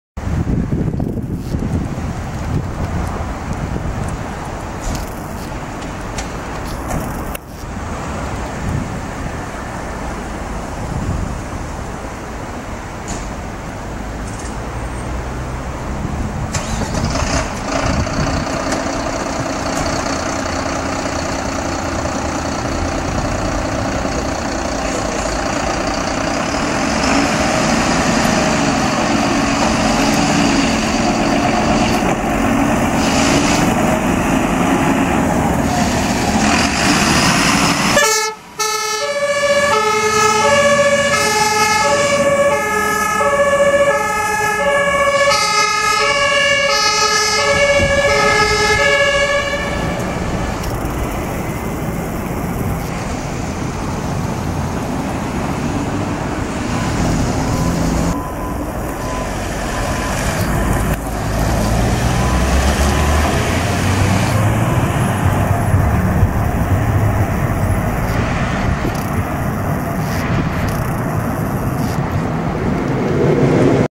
BF Hamburg, 22 HLF 1 + GW 22
Zu Sehen ist einmal der Gerätewagen 22 und das HLF 1 der BF Hamburg Fuer und Rettungswache 22 Berliner Tor.

Hamburg, Berliner Tor. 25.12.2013_______________________ Entspricht Aufnahmeort und Uplaoddatum

- §23 KunstUrhG -